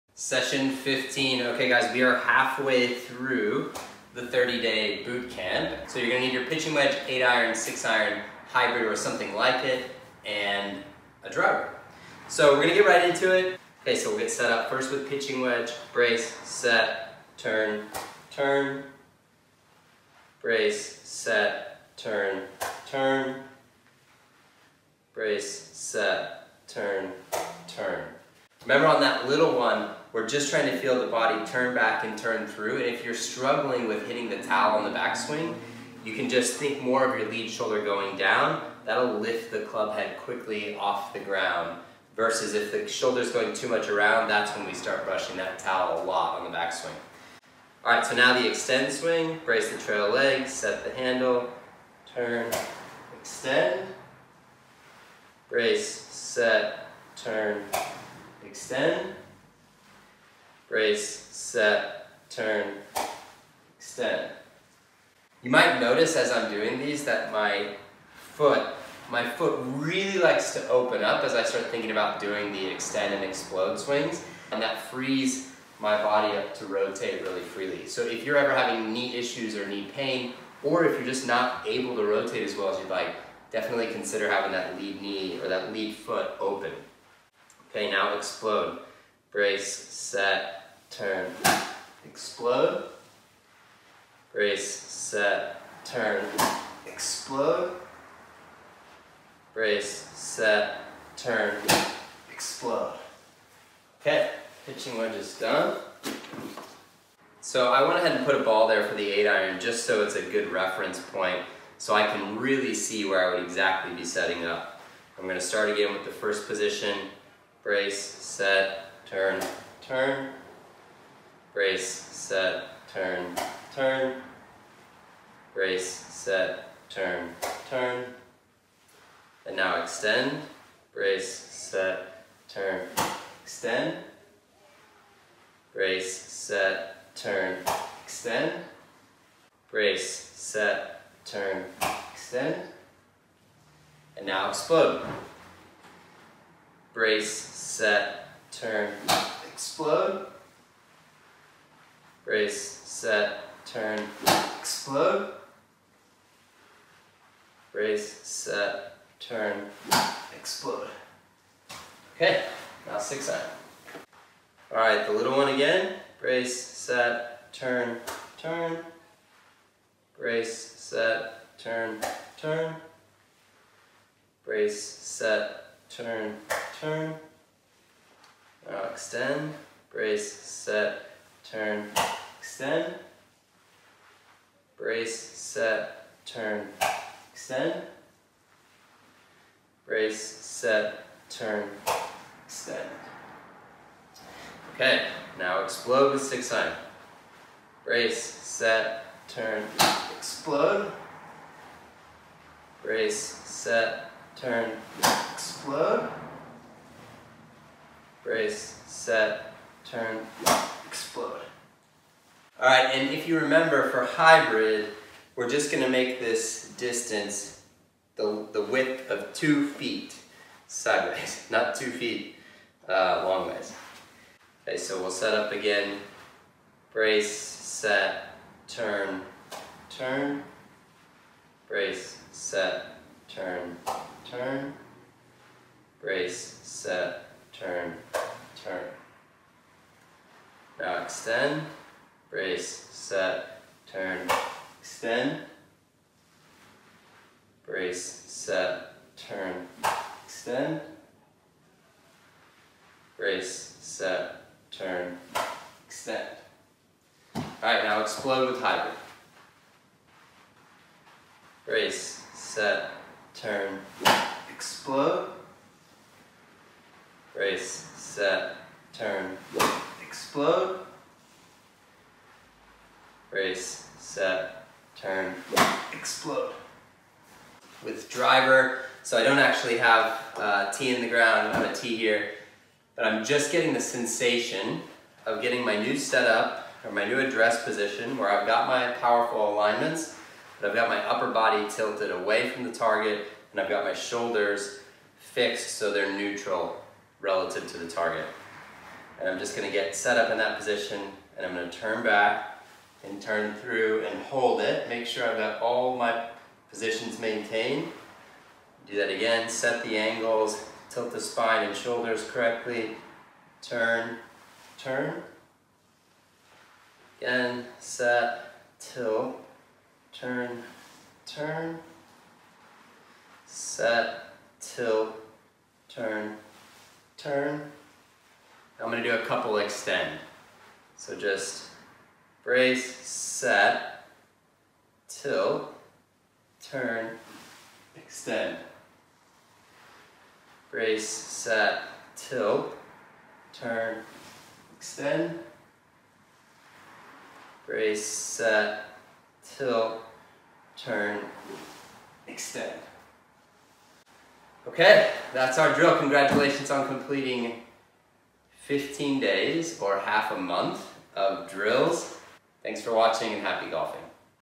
30 Day Bootcamp Session 15 Clips || Square to Square Swing
Going through the bag from PW to Driver (skipping a few) to master forward low point in the square to square swing.